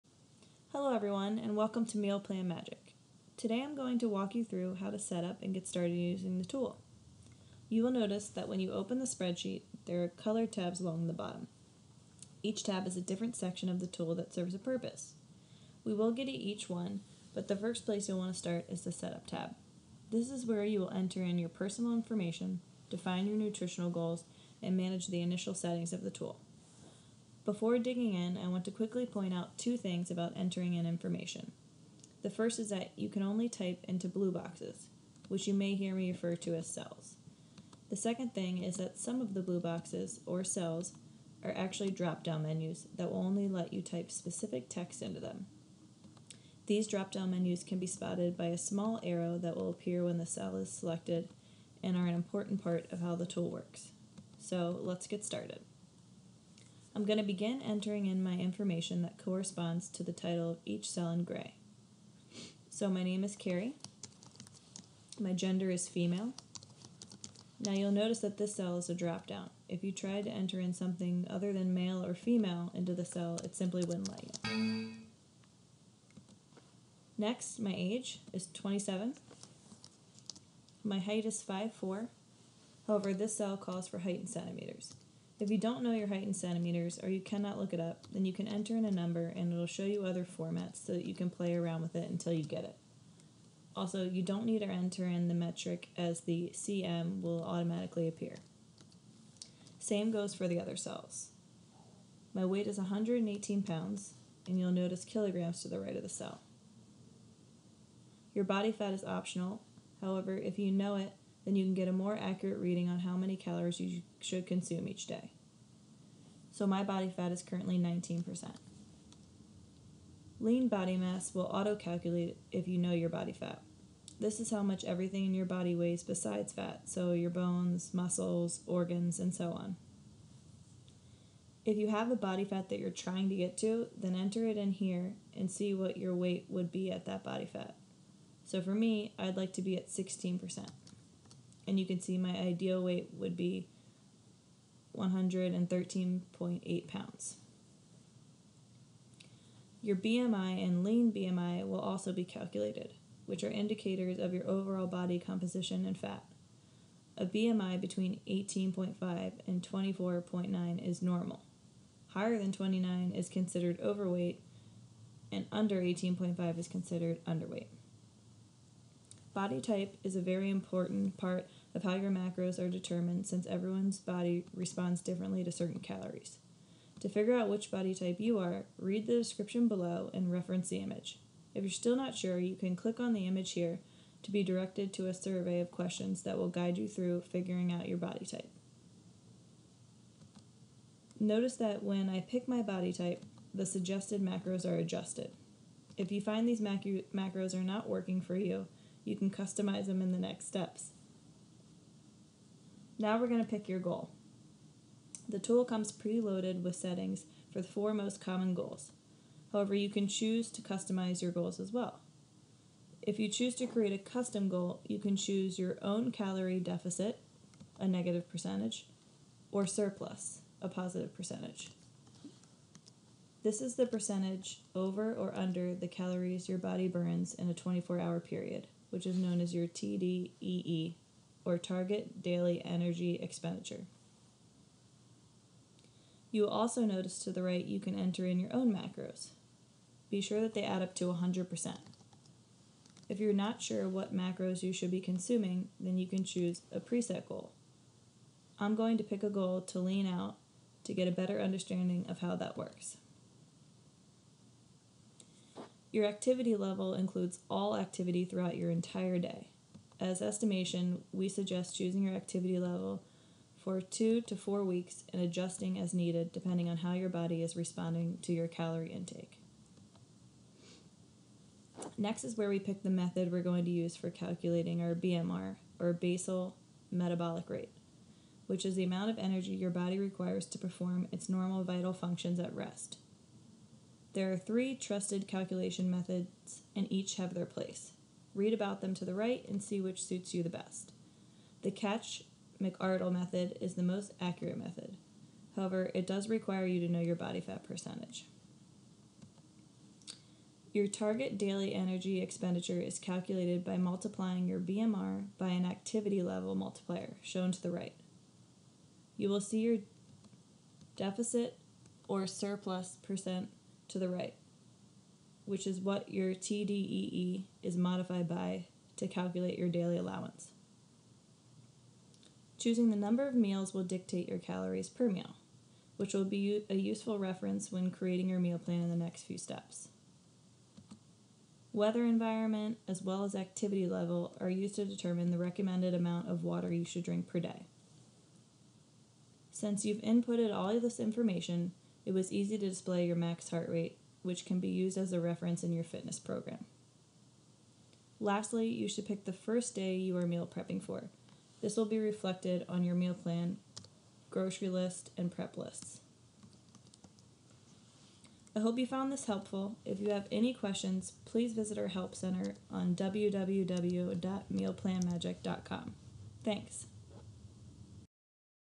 Get Setup
and our help center for questions.

Hello everyone and welcome to MealPlanMagic.
Today I am going to walk you through how to get set-up and start using the tool.
You will notice that when you open the spreadsheet, there are colored tabs along the bottom.  Each tab is a different section of the tool that serves a purpose.  We will get to each one, but the first place you want to start is the Set-up tab.  This is where you will enter in your personal information, define your nutrition goals, and manage the initial settings of the tool.
Before digging in, I want to quickly point out two things about entering in information.  The first is that you can only type into blue boxes, which you may hear me refer to as “cells.”  The second thing is that some of the blue boxes, or cells, are actually dropdown menus that will only let you type specific text into them.  These dropdown menus can be spotted by a small arrow that will appear when the cell is selected and are an important part of how the tool works.
I am going to begin entering in my information that corresponds to the title of each cell in grey.
My name is Keri.
My Gender is Female… Now you will notice that this cell is a dropdown.  If you tried to enter something other than Male of Female into this cell it would not let you.
My Age is 27.
My Height is 5’4’’, however this cell calls for height in centimeters.  If you do not know your height in CM or you cannot look it up…Then you can enter in a number and it will show you the other formats so that you can play around until you get it.  Also, you do not need to enter in the metric as the “cm” will automatically appear.
Same goes for the other cells… My weight is 118 lbs , and you will notice kilograms to the right of this cell.
Your body fat % is optional however if you know it then you can get a more accurate reading on how many calories you should consume each day.
My body fat is currently 19%.
 Lean Body Mass will auto-calculate if you know your body fat.  This is how much everything in your body weighs besides fat.  So your bones, muscle, organs, etc…
If you have a body fat that you’re trying to get to, then enter it here in order to see what your weight would be at that body fat. So for me, I would like to be at 16% and you can see my ideal weight would be 113.8 pounds.
Your BMI and Lean BMI will also be calculated, which are indicators of your overall body composition and fat.  A BMI between 18.5 and 24.9 is normal.  Higher than 29 is considered overweight and under 18.5 is considered underweight.
Body type is a very important in how your macros are determined since everyone’s body responds differently to certain calories.  To figure out which body type you are, read the description below and reference the image.  If you’re still not sure you can click on the image to be directed to a survey of questions that will guide you through figuring out your body type.
Notice that when I pick my body type, the suggested macros are adjusted.  If you find these macros are not working for you, you can customize them in the next steps.
Now we’re going to pick our goal. The tool comes pre-loaded with settings for the four most common goals, however you can also choose to customize your goals as well.